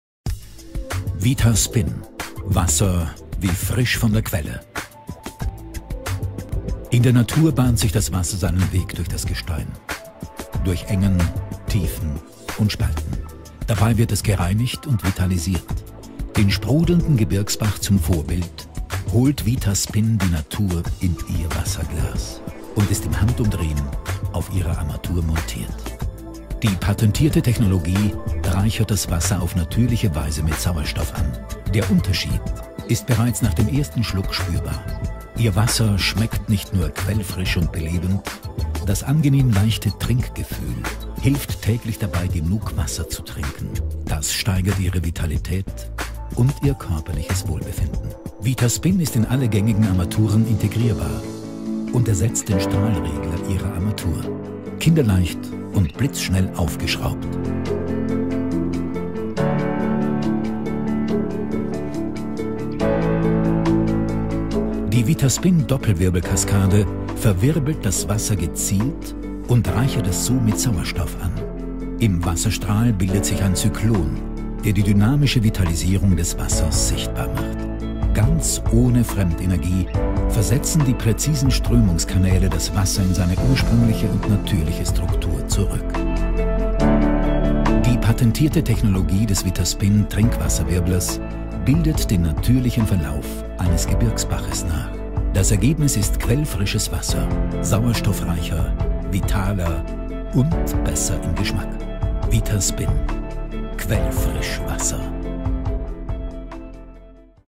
VITASPIN quell frisch wasser
VITASPIN - QUELL FRISCH WASSER - WIRD NICHT MEHR PRODUZIERT !!

Mit Vitaspin erhalten Sie im Handumdrehen quellfrisches Wasser. Das verwirbelte Wasser schmeckt nicht nur besser, das angenehm leichte und frische Trinkgefühl hilft täglich dabei, genug Wasser zu trinken. Das steigert Ihr körperliches Wohlbefinden auf ganz natürliche Weise.

"Alles Leben entspringt aus dem Wasser. Das Wasser ist demnach die eigentliche Lebensquelle. Grund genug, sich mit dieser genau zu befassen."
(Viktor Schauberger)

Wasser ist eine essentielle Voraussetzung für das Leben auf der Erde. Von Natur aus ist es lebendig und frisch. Es verlässt seine Quelle in optimaler Qualität. Doch bis es zu uns gelangt, ist es zahlreichen Belastungen ausgesetzt und verliert seine ursprüngliche Vitalität und Kraft. So ist unser Leitungswasser zwar trink- und genießbar - den Geschmack von frischem Quellwasser hat es jedoch längst verloren.

Ein dynamischer Prozess, die so genannte Wasserverwirbelung ist nun in der Lage, dem Wasser seine ursprüngliche Kraft und Vitalität wieder zurückzugeben. Das Wasser wird revitalsiert, quellfrisch im Geschmack.

Den sprudelnden Gebirgsbach zum Vorbild

Auf kleinstem Raum saugt das Wasser den Sauerstoff in der Vitaspin-Doppelwirbelkaskade Ihrer Armatur an. Nach dem Vorbild der Natur: Dort bahnt sich das Wasser seinen Weg durch das Gestein -- durch Engen, Tiefen und Spalten.

Dabei wird es gereinigt und vitalisiert. Im Wasserstrahl Ihrer Armatur bildet sich ein Zyklon, der diese dynamische Vitalisierung des Wassers nachbildet. Ganz ohne Fremdenergie, nur mit dem Wasserdruck in der Leitung, revitalisiert sich Ihr Trinkwasser auf natürliche Weise.